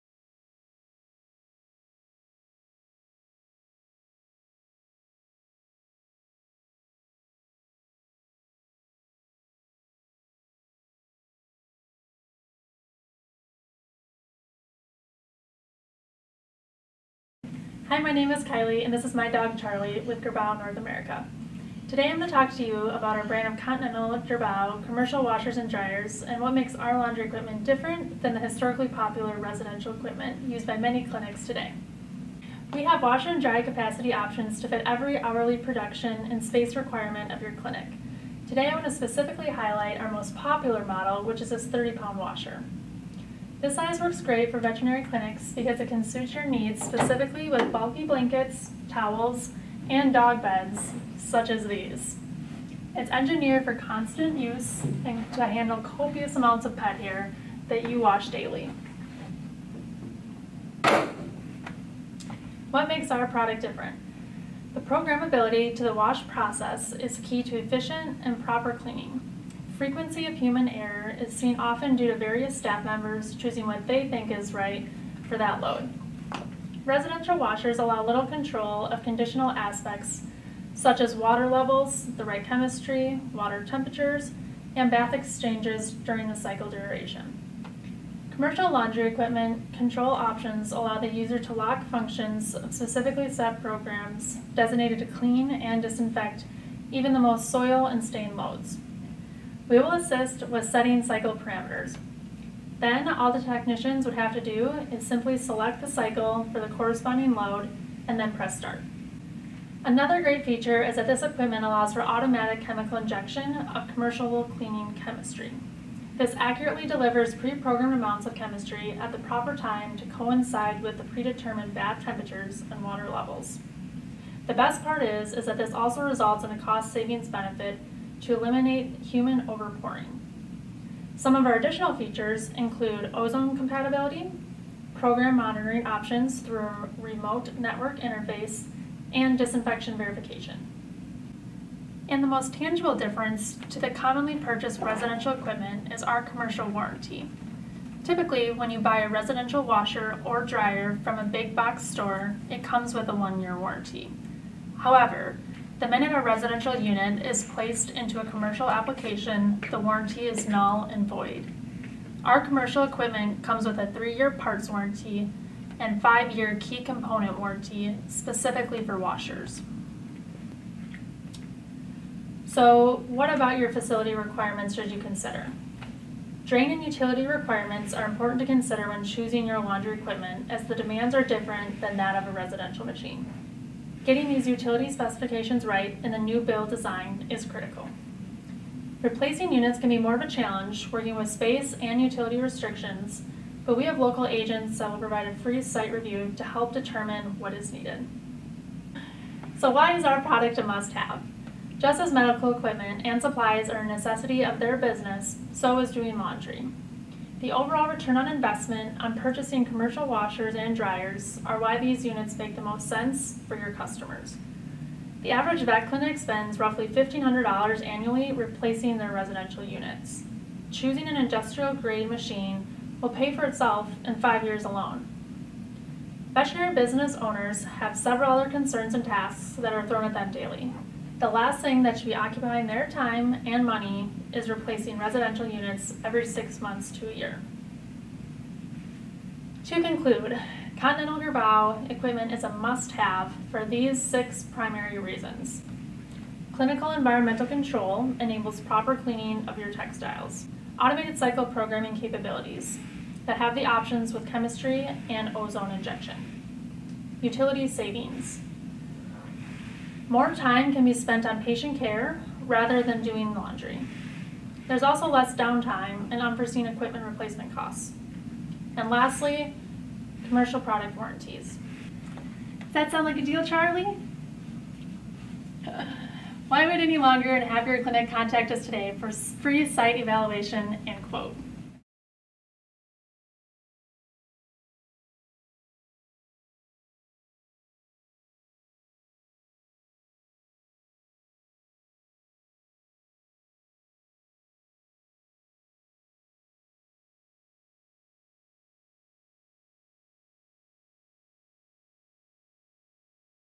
Girbau Commercial Washer and Dryers
Girbau commercial washer and dryers are engineered to drastically improve laundry productivity, efficiency and cleanliness. Thus, your veterinary practice completes more laundry in less time —using less labor and energy.